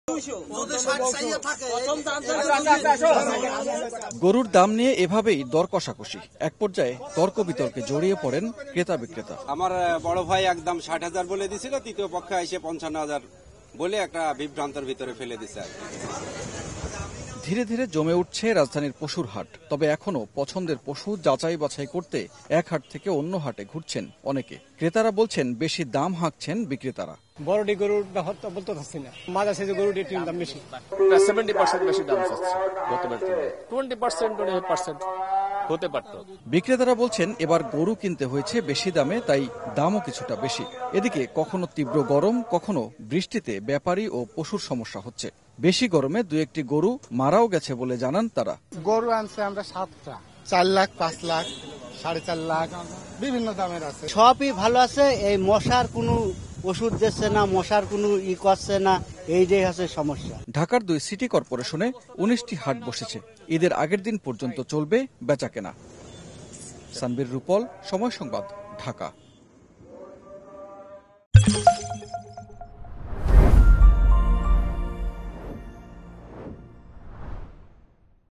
রাজধানীর পশুর হাটে বাড়ছে ক্রেতা সমাগম। | Dhaka Cow Haat | Qurbani Cow | Somoy National
রাজধানীর পশুর হাটে বাড়ছে ক্রেতা সমাগম। | Dhaka Cow Haat | Qurbani Cow | Somoy TV

জমে উঠছে রাজধানীর পশুর হাট। বাড়ছে ক্রেতা সমাগম। কাল থেকে পুরোদমে বেচাকেনা বাড়ার আশা বেপারিদের। এদিকে, এখনো ট্রাকে ট্রাকে আসছে গরু। দাম চড়া বলছেন ক্রেতারা।